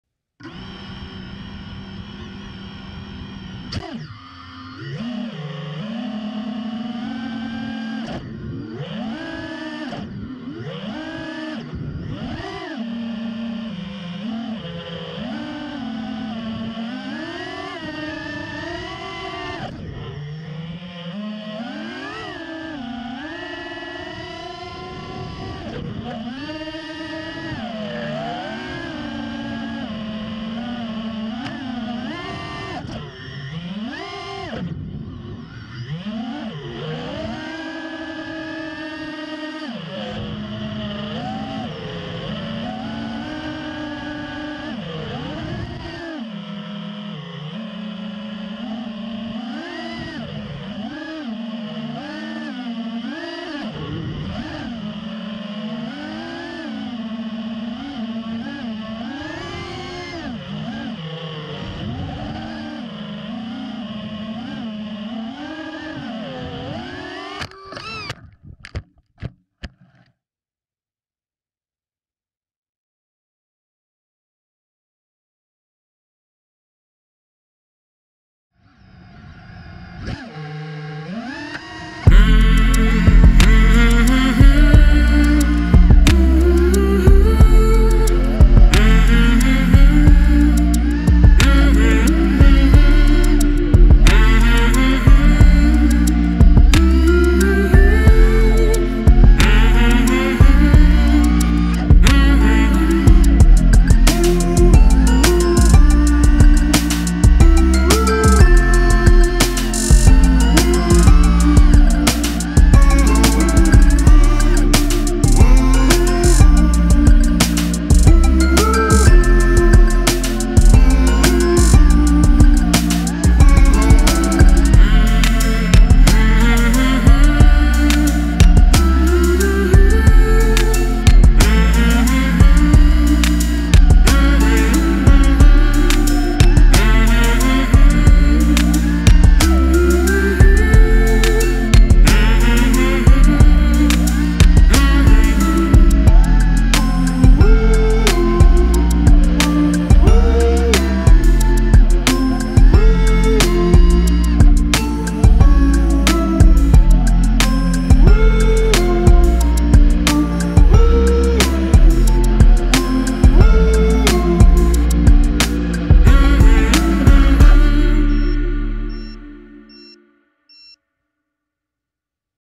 TBS Source One
The Quad
Frame: TBS Source One
Flight controller: Mamba F405
ESC's: Mamba F40HV ESC (4 in 1)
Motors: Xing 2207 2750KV
Camera: Caddx Ratel
VTX: Eachine TX805
Antenna: iFlight pagoda
RX: XM+
Lost model buzzer: Finder JHE42B
HD: Foxeer Box 2